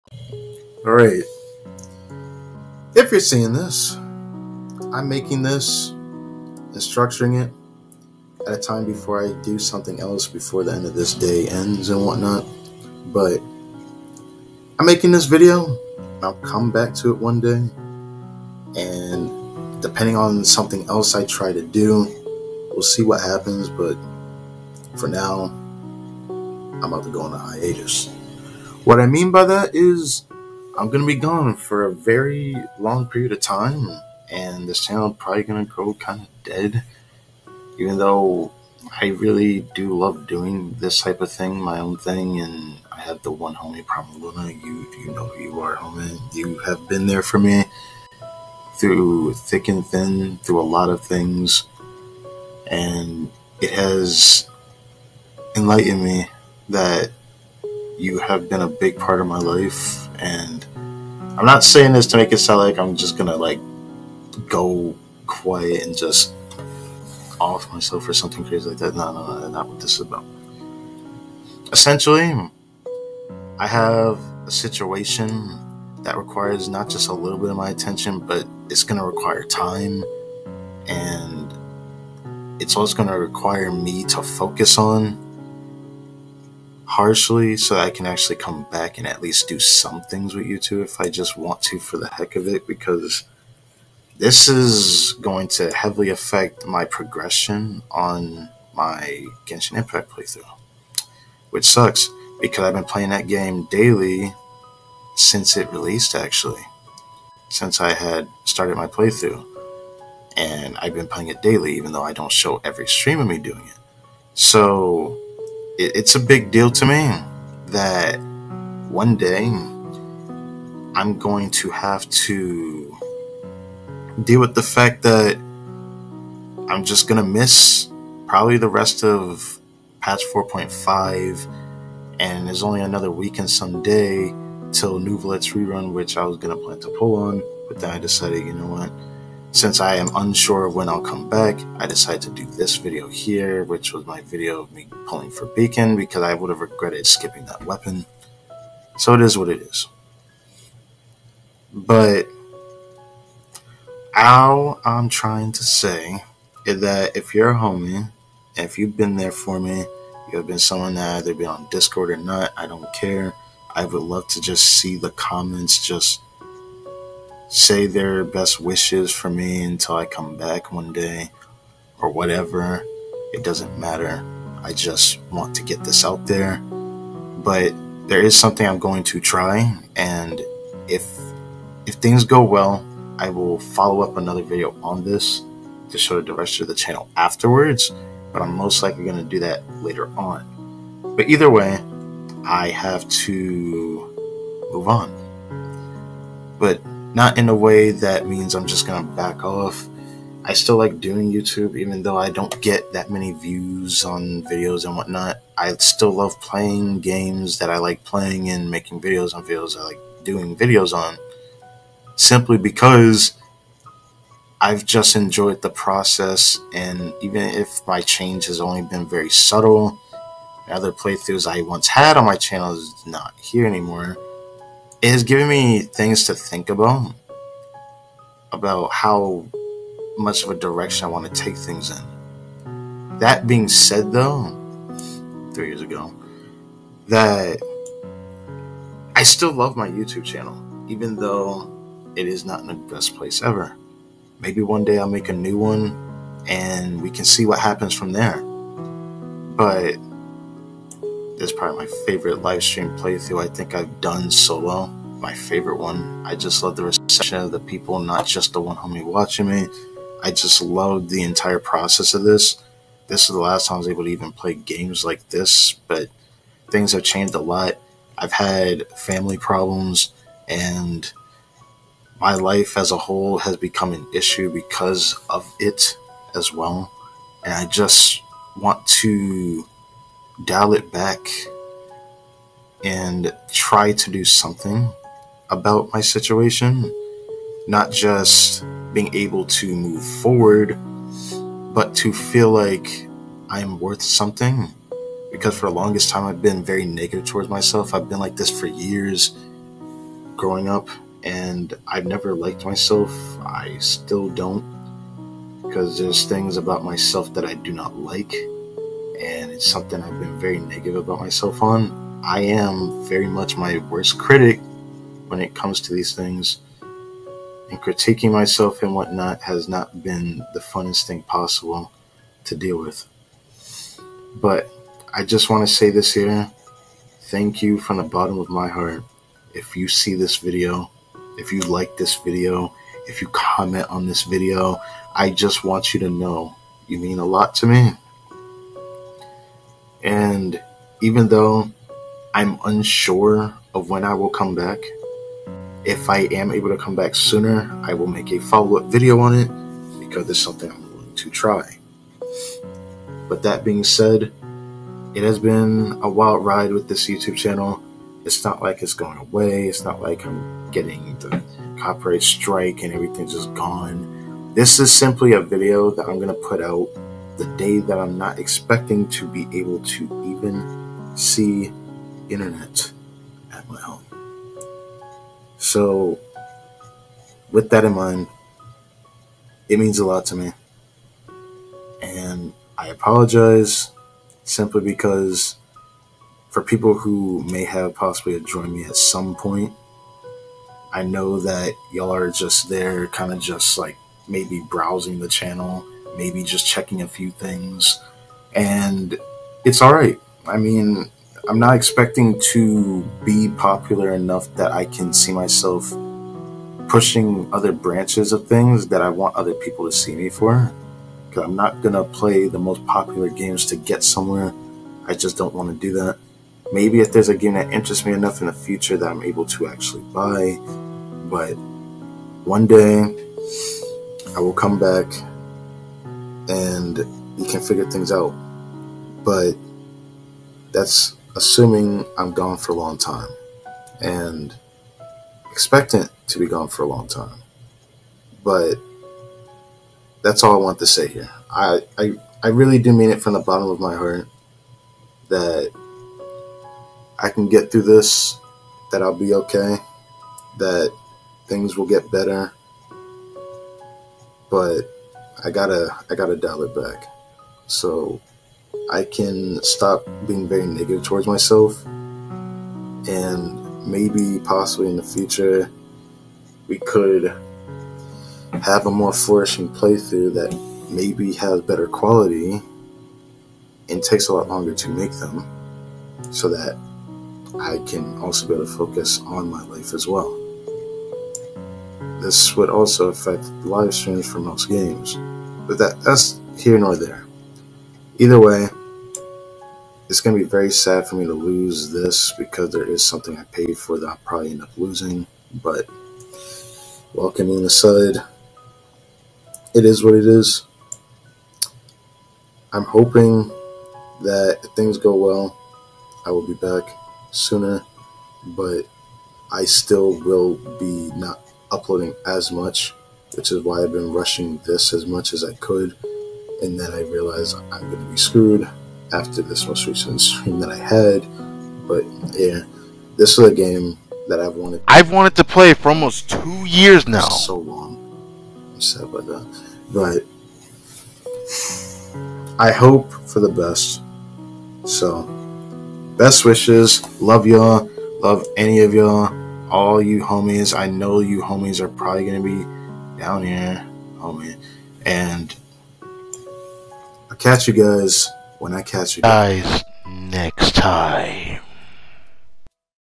A Homie's Gotta Do What A Homie's Gotta Do (I'll Be Back...)
There are just things I feel like needs to be done in my personal life so I have to change. In the face of a really messed up situation I decided to make this video regardless of what's going on atm but I plan to try something so If I come back sooner than lets say another full month from now or under I'll do a follow up video explaining how I'll treat my youtube channel from this year onward. Love y'all who have been there for me on discord and you primalluna you mean a lot to me more than you know. I hope the people who watch me will still be here when I return and If not I understand...

I originally planned to do a face reveal but I'll save it for another time